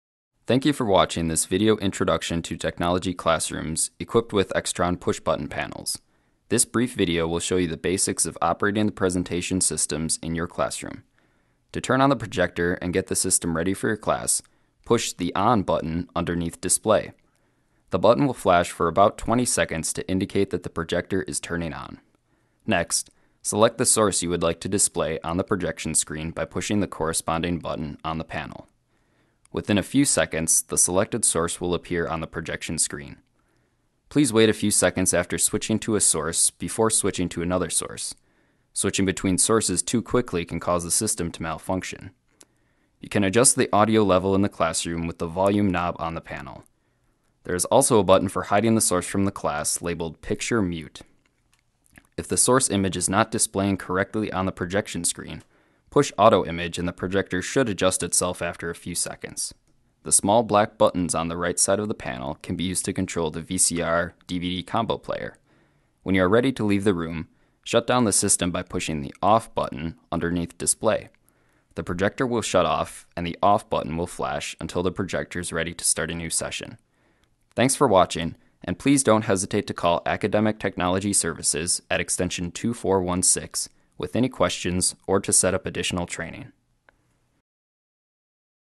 Controlling Classroom Technology With Extron Push-Button Controls
Instructional video to demonstrate the control of classroom technology in UWGB classrooms using Extron Push-Button controls.

Voiceover and Hand Modeling by Sam Owens
Script, Camera, and Edit by Scott Berg